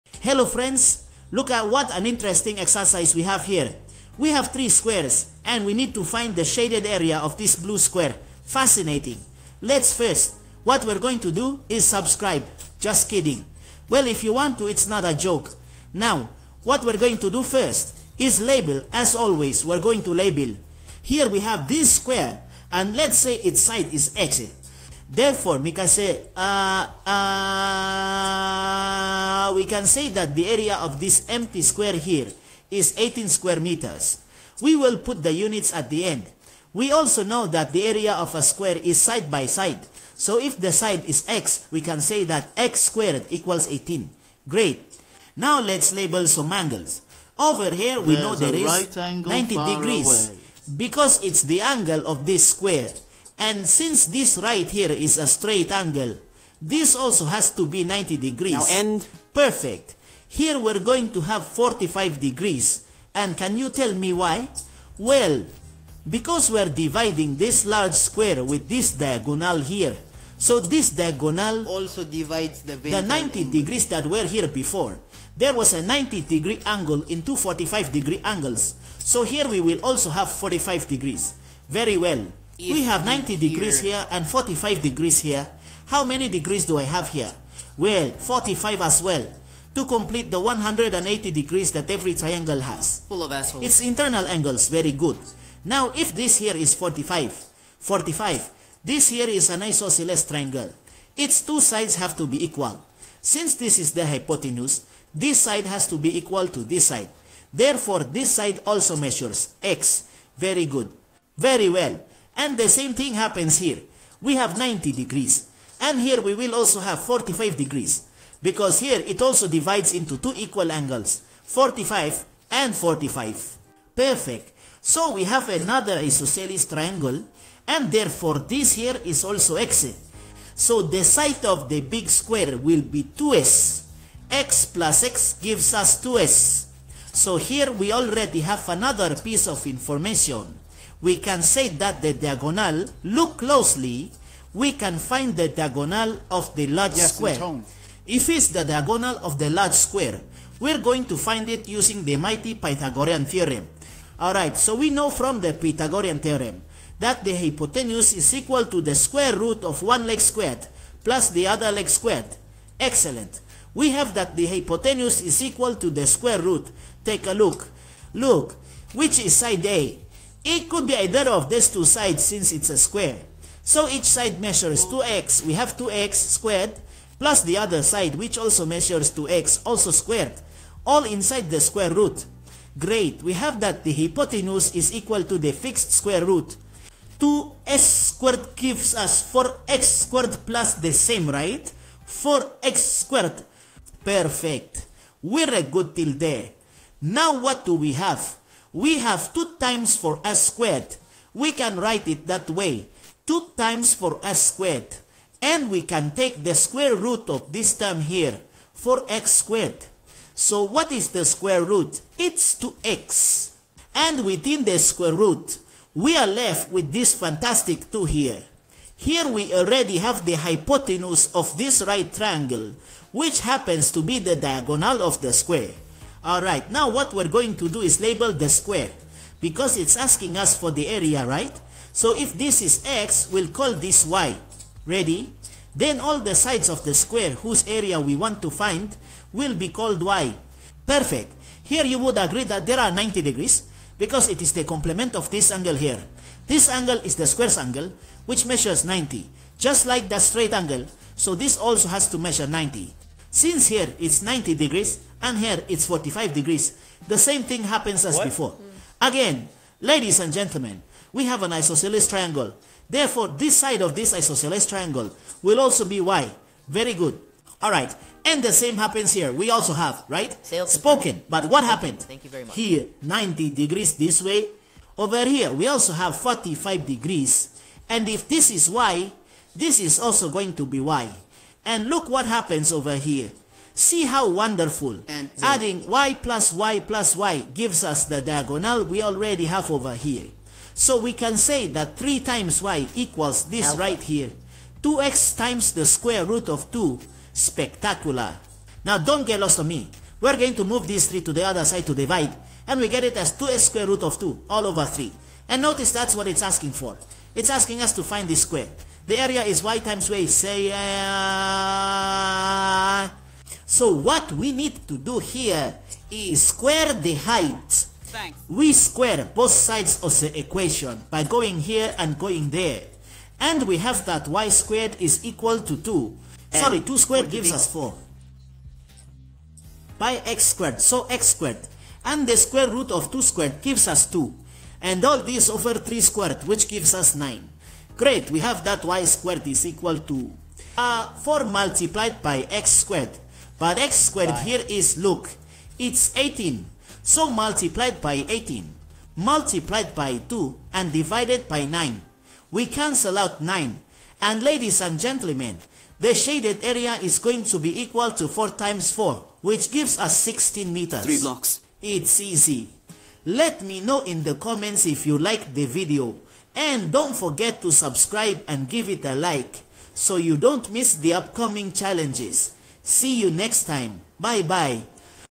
⚡Lightning Challenge⚡ What is the AREA OF THE SQUARE
⚡Lightning Challenge⚡
 Hello friends ! You can't miss this Fascinating Shaded Areas Challenge.
😀😀 I hope these videos help you and inspire you to study 😀😀

Friends, apologies for the audio, it is created with AI because I can't speak English. Eventually I will learn the language and upload videos with my voice.